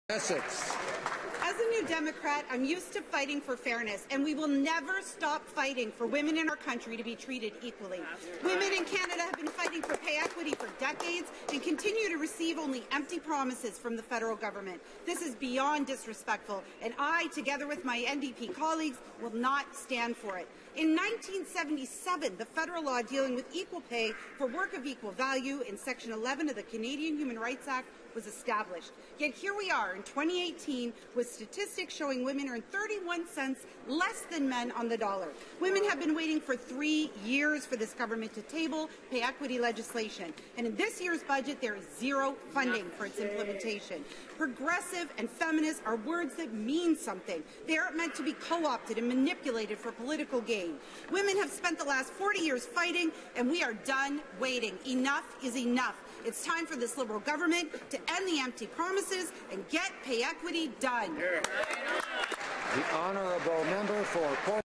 Tracey demands Liberal action on pay equity legislation
As a New Democrat, I am used to fighting for fairness and we will never stop fighting for women in our country to be treated equally.

  Women in Canada have been fighting for pay equity for decades and continue to receive only empty promises from the federal government. This is beyond disrespectful and I, together with my NDP colleagues, will not stand for it.

  In 1977, the federal law dealing with equal pay for work of equal value in Section 11 of the Canadian Human Rights Act was established, yet here we are in 2018 with statistics showing women earn 31 cents less than men on the dollar.

  Women have been waiting three years for the government to table pay equity legislation and in this year's budget there is zero funding for its implementation.

  Progressive and feminist are words that mean something. They are not meant to be co-opted and manipulated for political gain.

Women have spent the last 40 years fighting and we are done waiting. Enough is enough. It is time for the Liberal government to end the empty promises and get pay equity done.